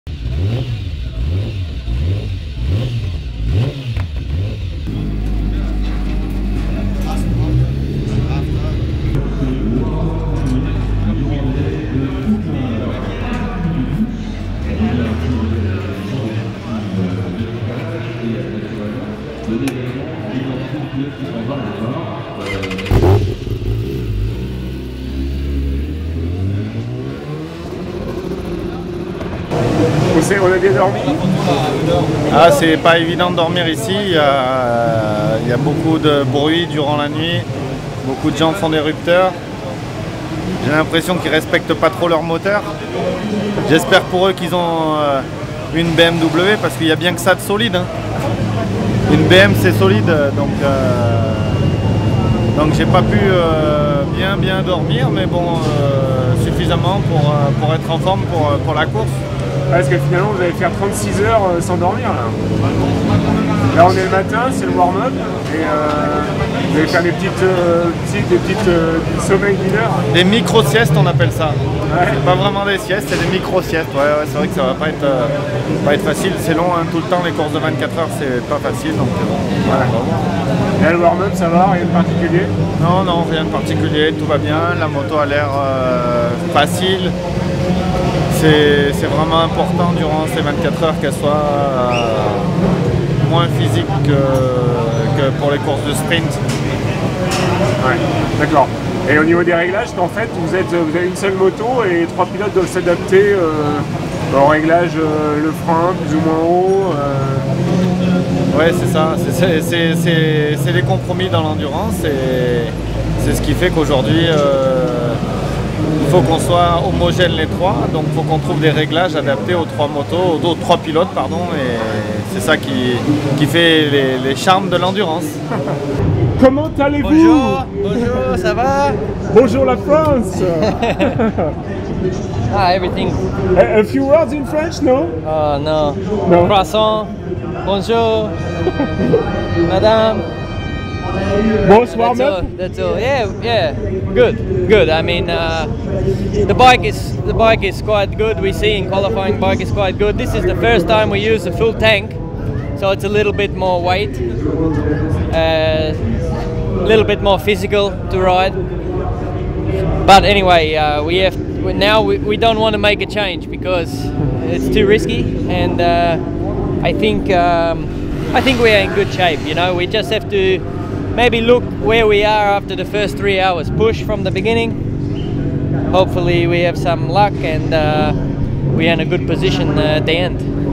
24H Moto - Le Mans 2012 - Warm UP - Erwan Nigon et Damian Cudlin
Interview exclusive des pilotes du Team BMW Motorrad France, Erwan Nigon et Damian Cudlin à l'issue du warm up !